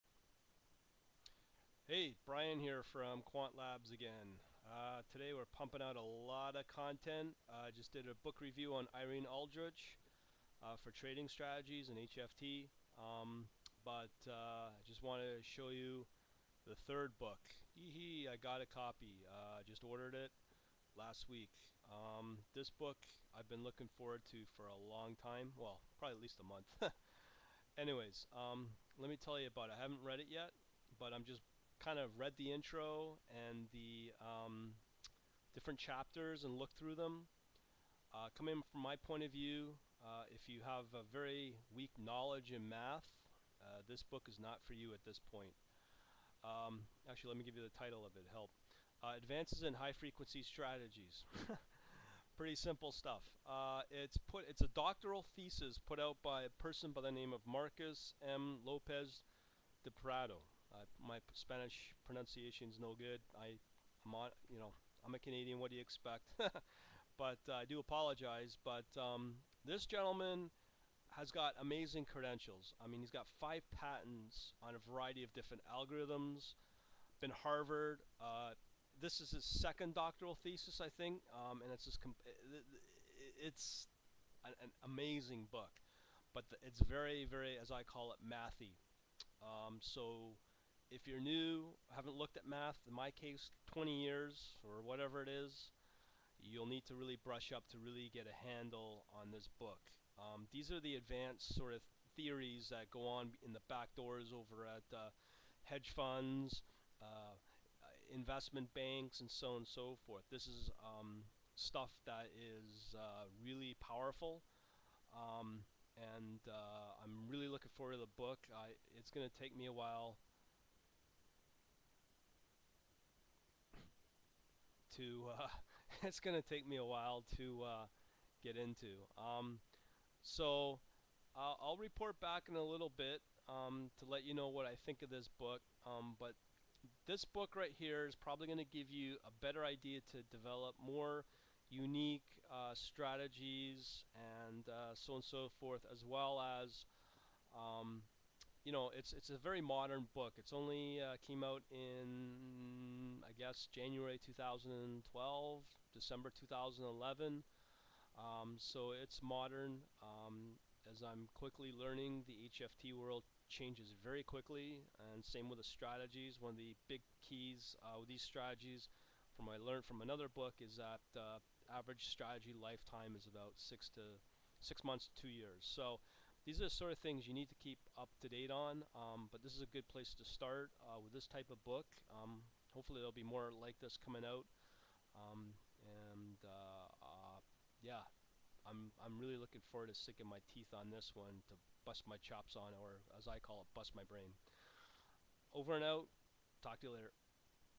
Advances in High Frequency Strategies HFT Quant Book Review
I talk about this most sophisticated book I have yet to read. It is about advanced HFT strategies with complex math to go along. At this  point, the graphs look pretty in black and white.